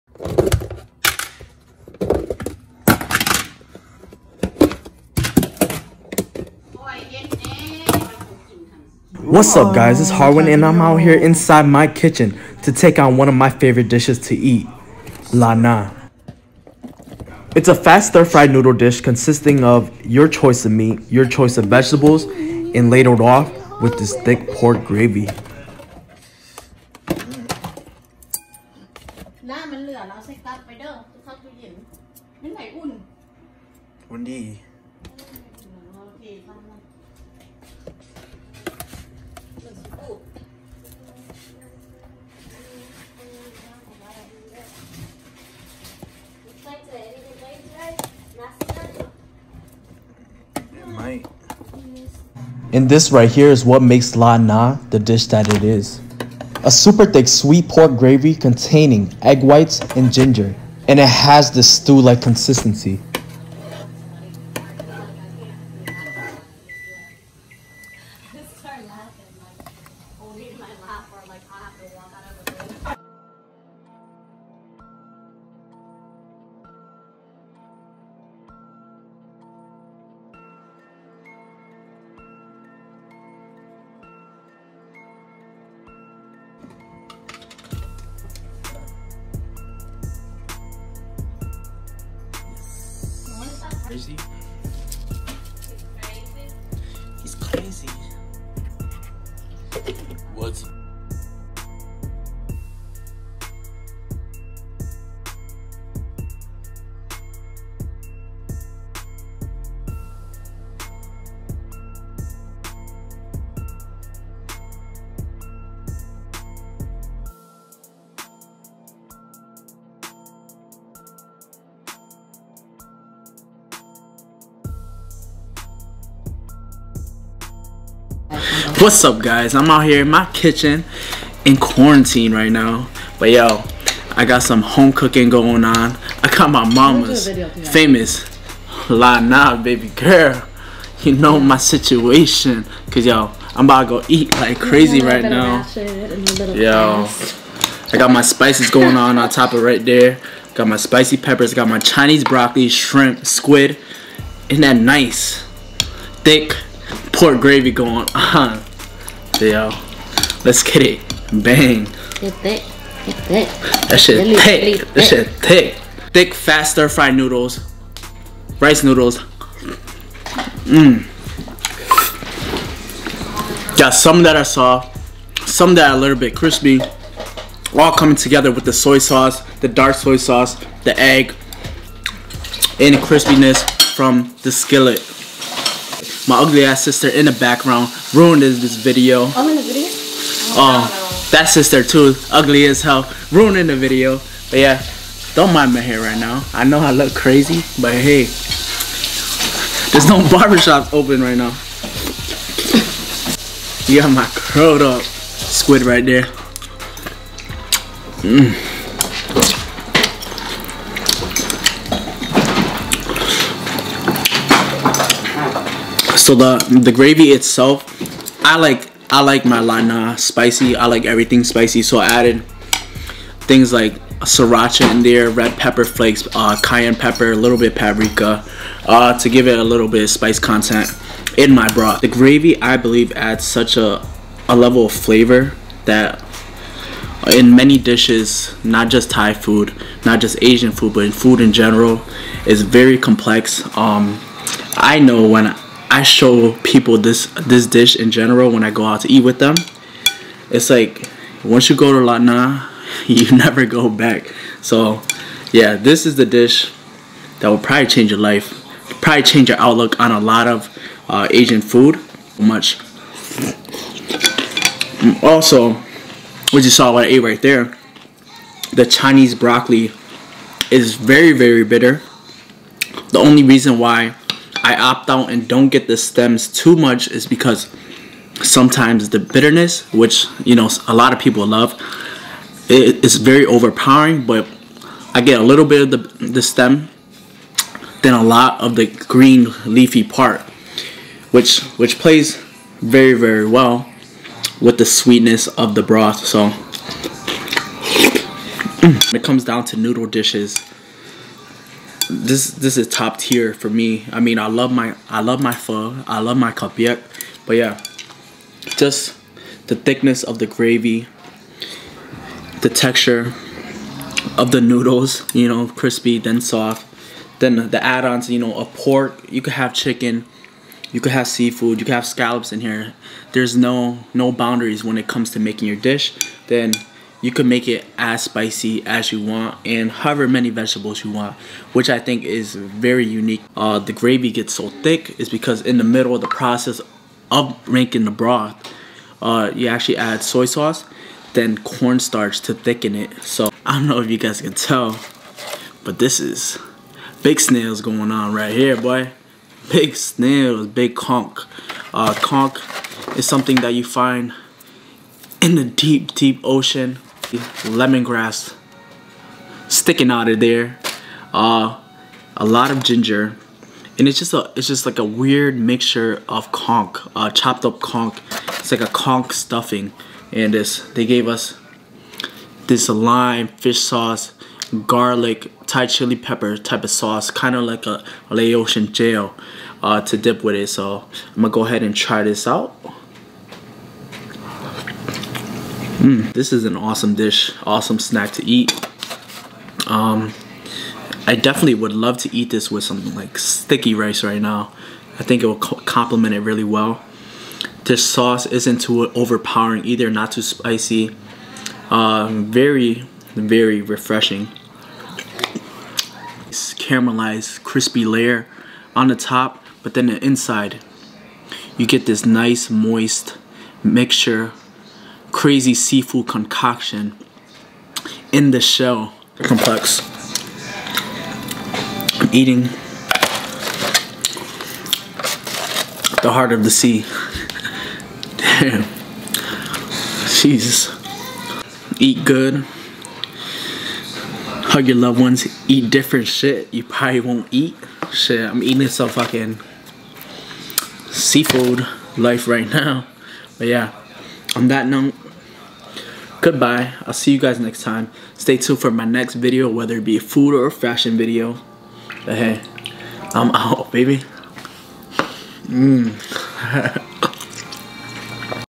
SapSap Kitchen: Seafood Lad Na Homemade Style!
What’s up guys I’m here inside my kitchen eating one of my favorite dishes. Watch as I grub on and devour this delicious homemade feast. And oh wait yes there’s fat ass snails in this video too!